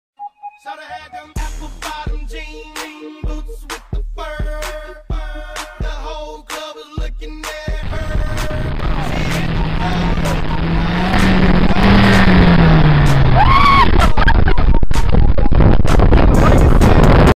Course des voitures - FSEG Sousse
BMW 316i VS Clio
Majd et Bouraoui